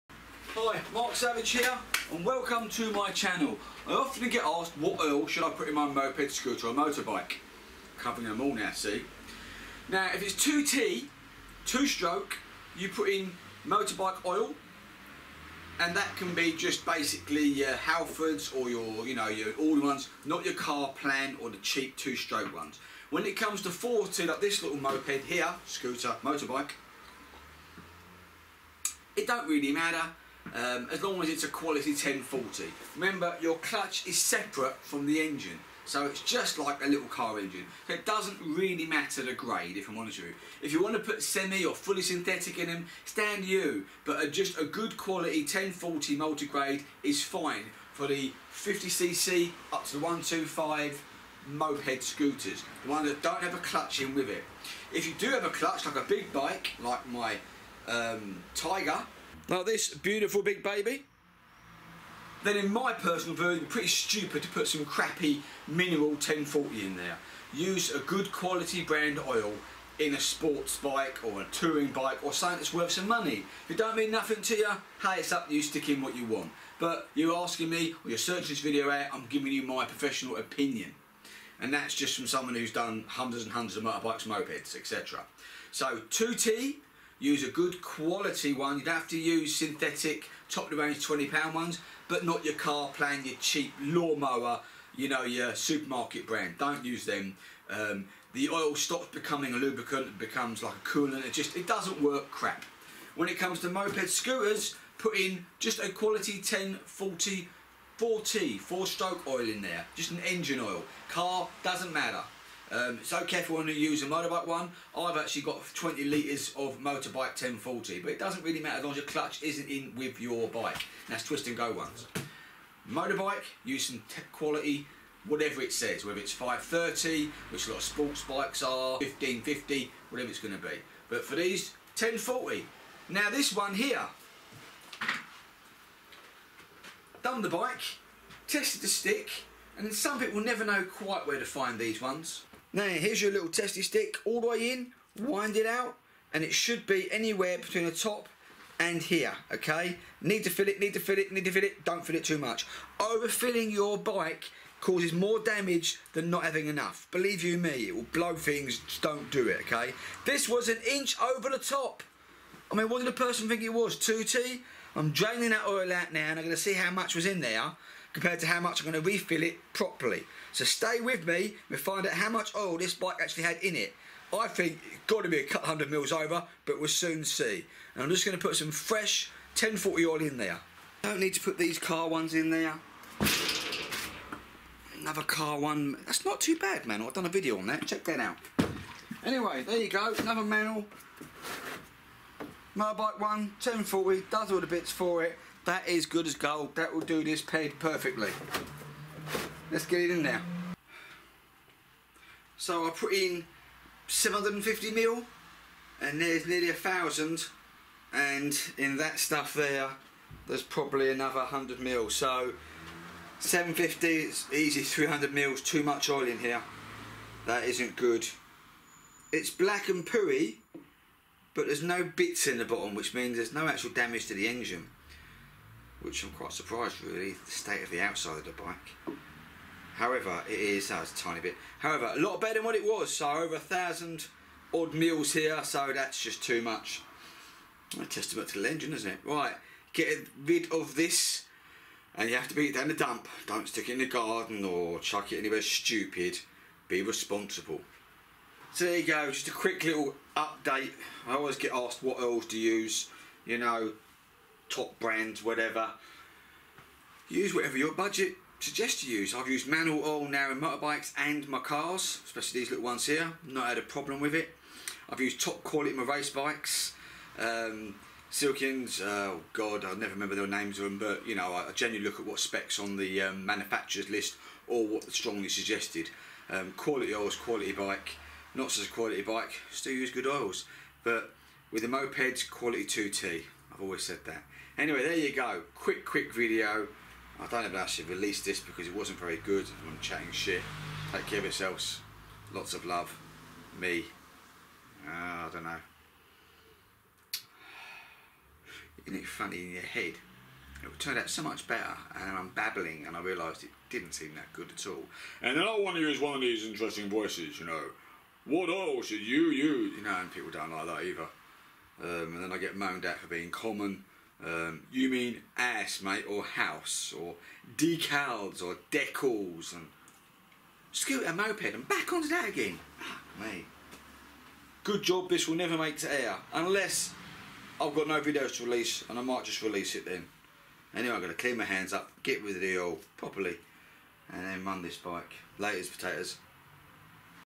WHAT OIL SHOULD I USE IN MY MOTORBIKE MOPED SCOOTER 2T 4T
SIMPLE & QUICK VIDEO ON THE OIL TO USE
ME MOANNIG ,SOMETIMES ITS JUST ME IN MY GARAGE AND I GO A LITTLE MAD PMSL